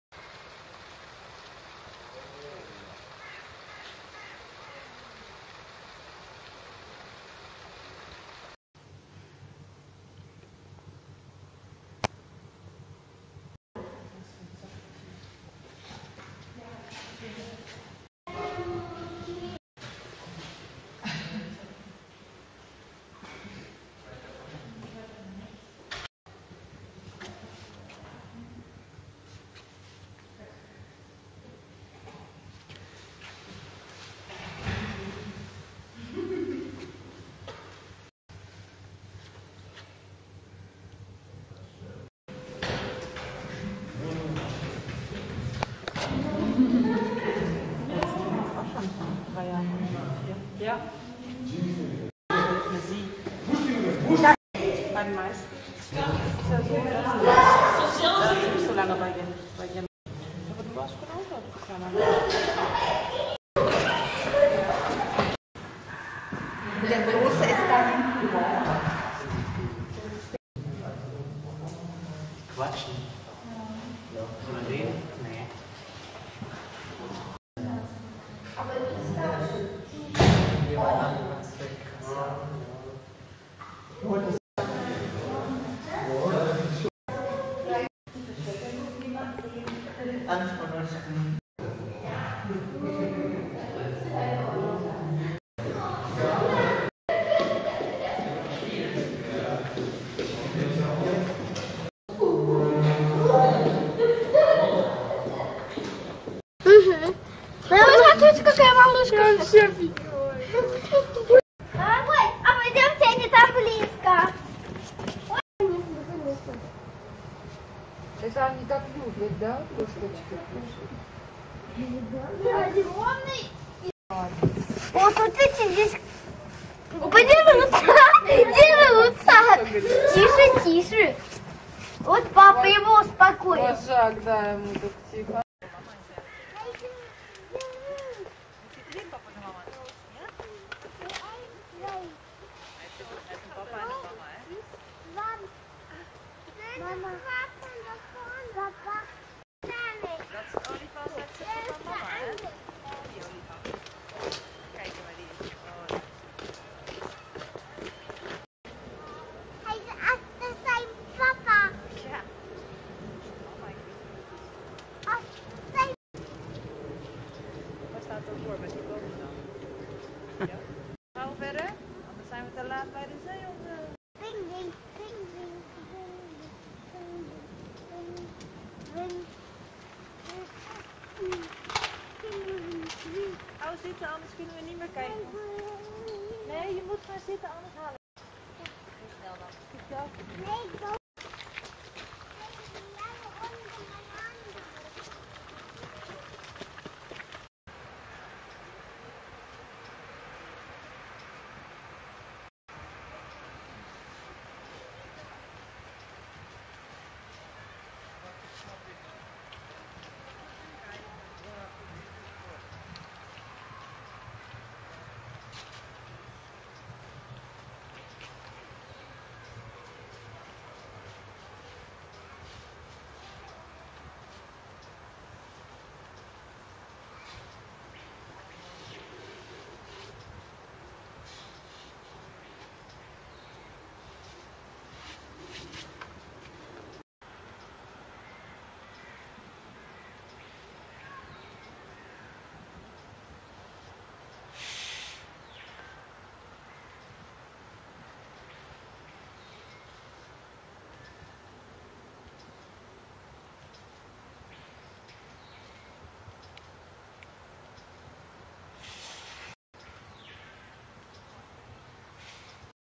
Little Elephant Ludwig Climbs On Top of Mother Temi-Orang Utan Family-Newcomer Yahe-Pavians
Elefant Ludwig klettert auf Mama Temi's Rücken - Orang Utan Familie Bruno - Matra - Jolie und Newcomer Yahe - Paviane -  Tierpark Hellabrunn